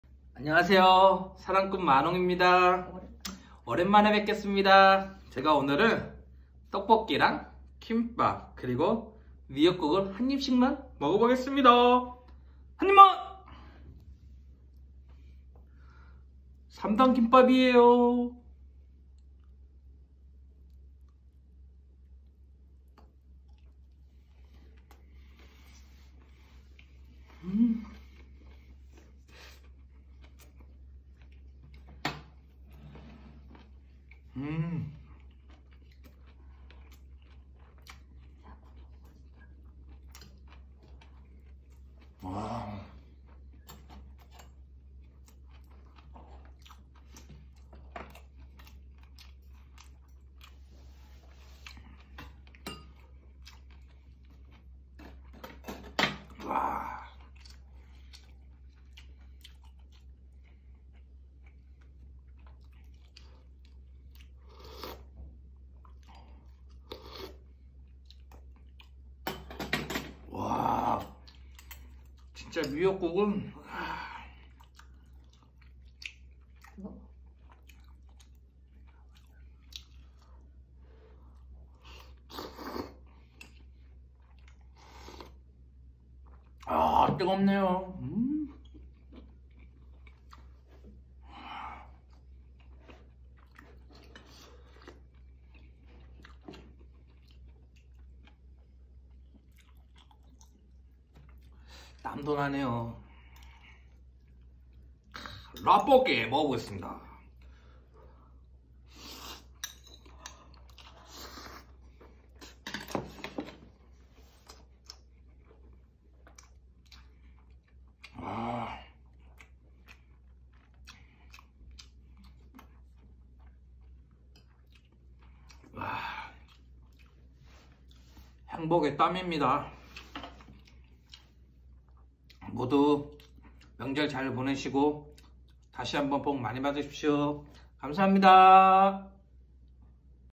(먹방)김밥 라볶이 미역국 한입만 행복의땀!!
오랜만에!! 업로드 했습니다!!!
직접만든 라볶이 김밥 미역국!! 너무 맛있네요!!!
라면 사리는!! 비빔면 라면인데 진짜 너무 맛있네요!
감기 조심 코로나 조심 항상 행복하기!!!